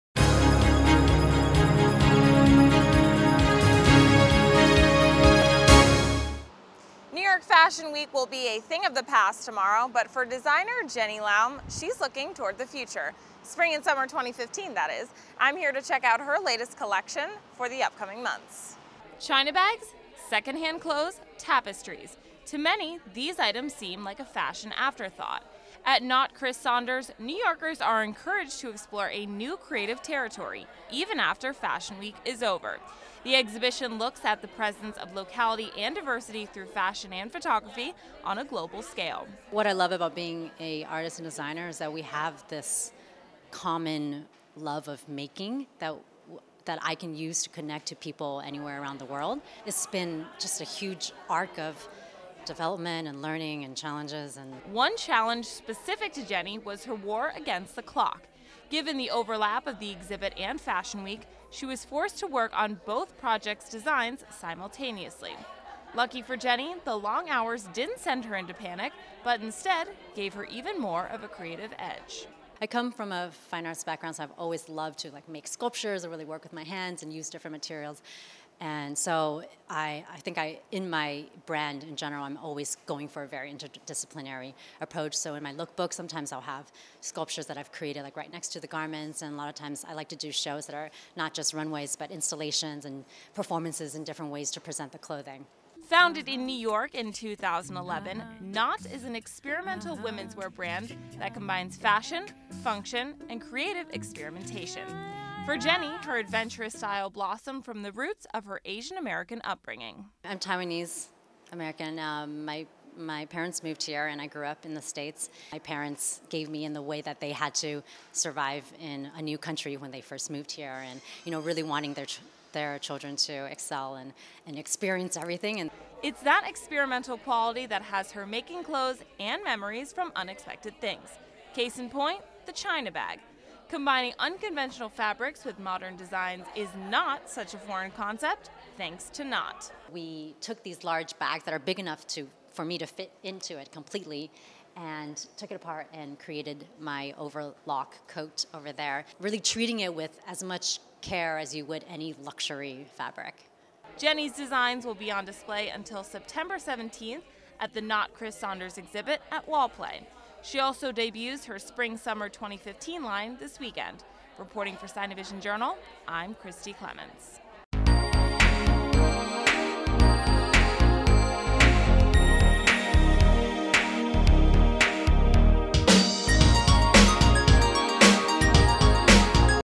Hey, Hey, it's Jenny Lai!
Lincoln Center has quieted down now that Fashion Week is over. For Asian American designer, Jenny Lai, she’s looking to pump up the volume with her latest exhibit NOT X Chris Saunders. It’s NOT what you’d expect! SinoVision Journal reporter Christie Clements caught up with her.